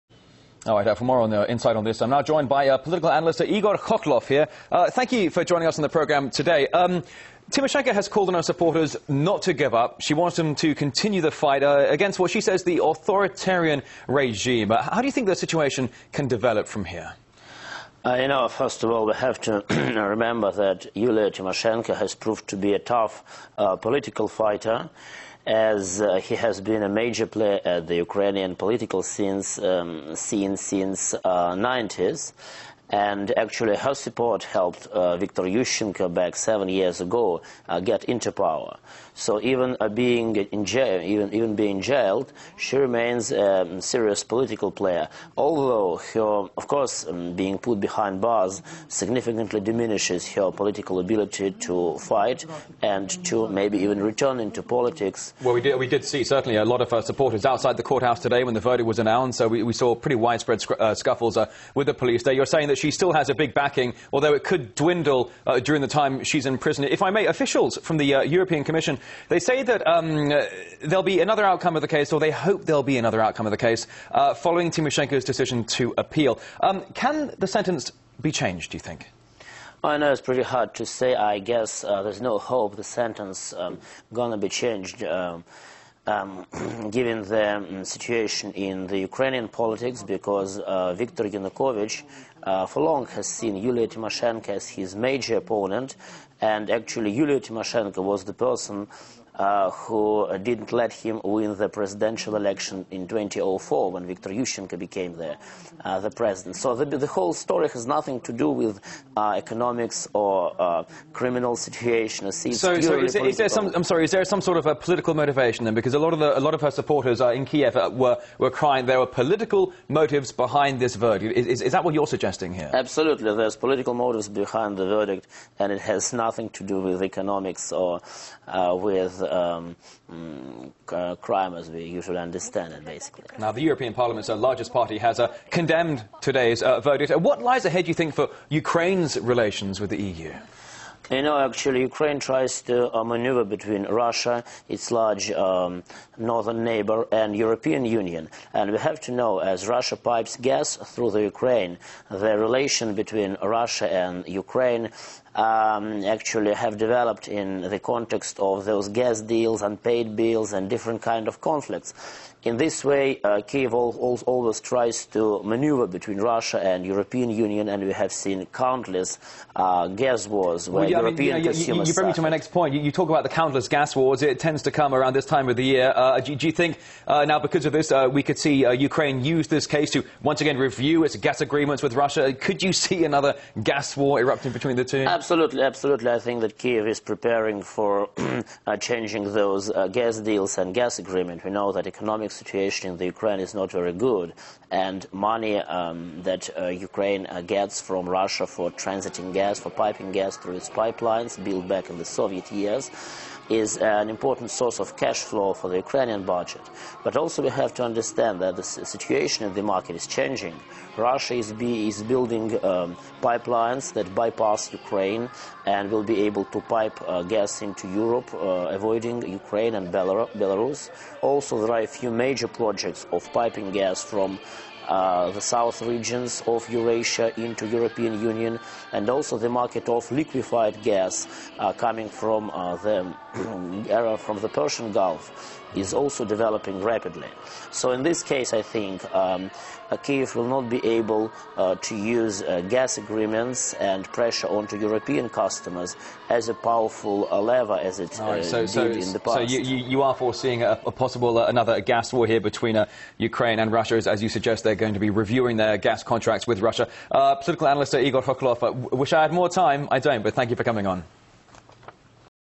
Yulia Tymoshenko being jailed for gas deals with Russia
Political analyst Igor Khokhlov told during his TV interview he believes the sentence for Yulia Tymoshenko who had been being jailed for gas deals with Russiais unlikely to be changed.

"I guess there is no hope the sentence is going to be changed given the situation in Ukrainian politics, because Viktor Yanukovich has for a long time seen Yulia Tymoshenko as his major opponent and actually Yulia Tymoshenko was the person who did not let him win the presidential elections 2004, when Viktor Yushchenko became president there," Khokhlov said.

He agrees the case has nothing to do with economics or crime, and says there are political motives behind the verdict.